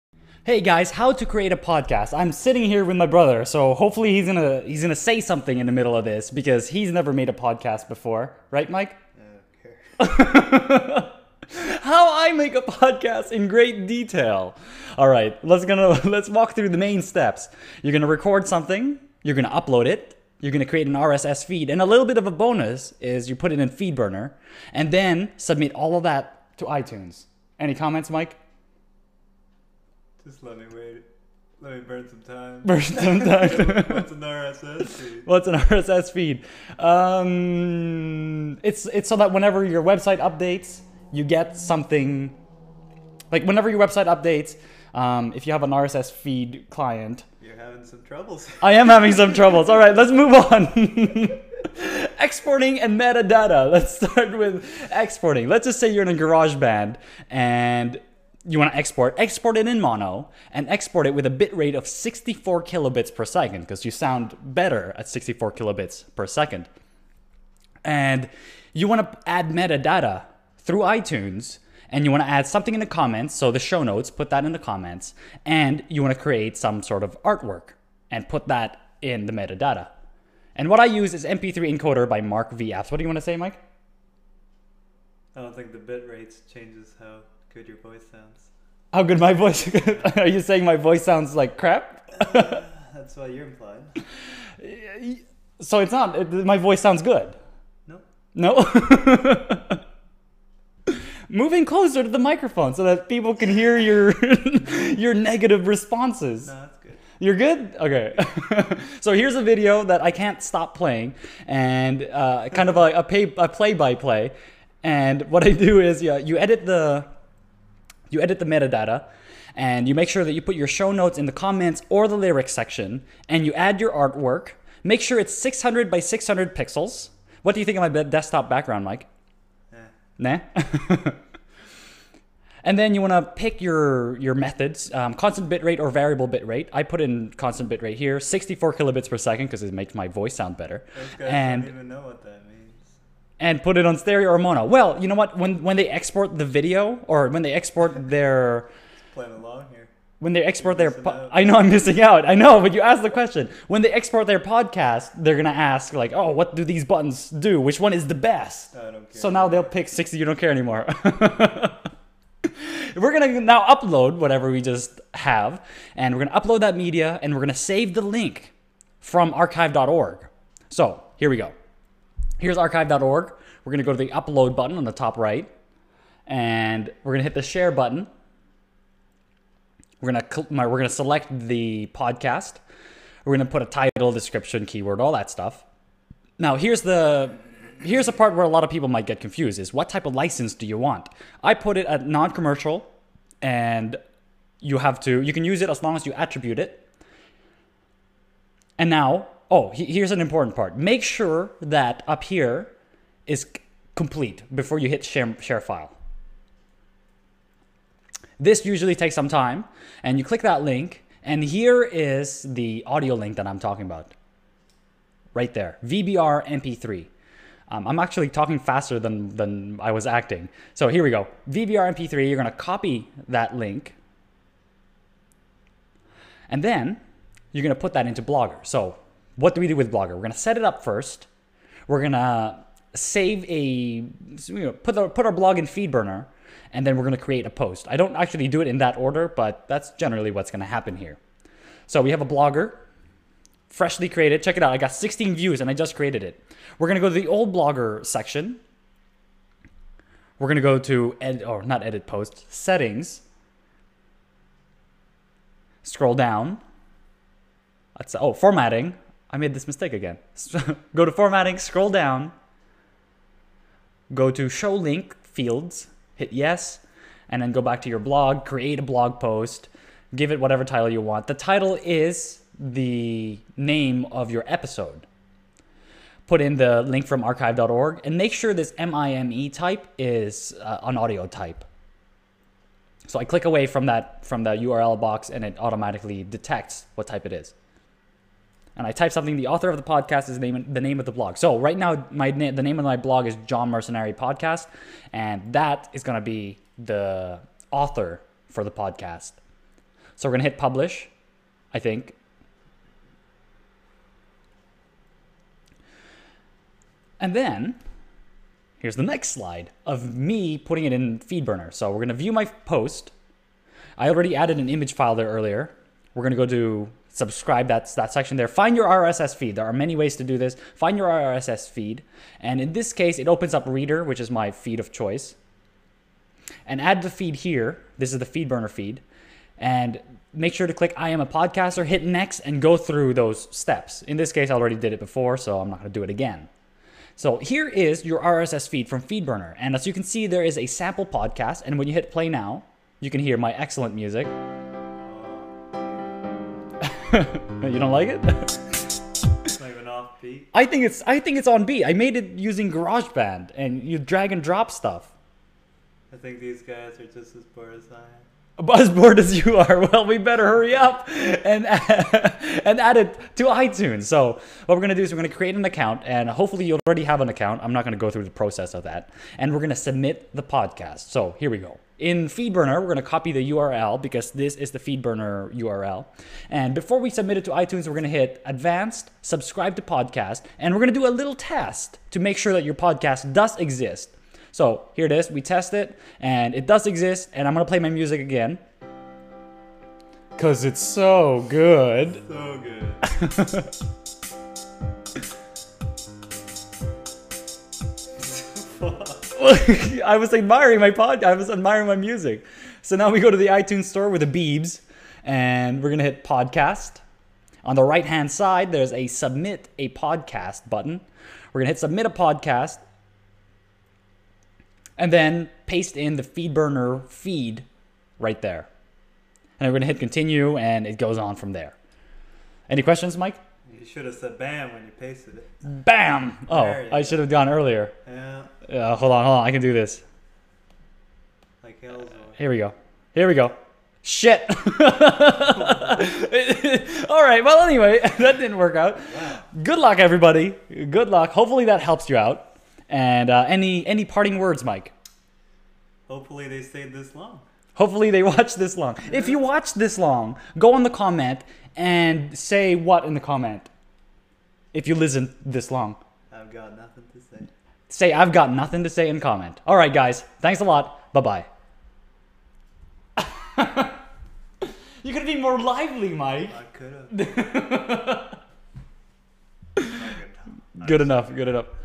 How to make a podcast. Exporting, Uploading, RSS, iTunes
This is only one of many ways to make it.

-Creating an RSS feed
-Creating a feedburner feed
-Submitting your podcast to iTunes.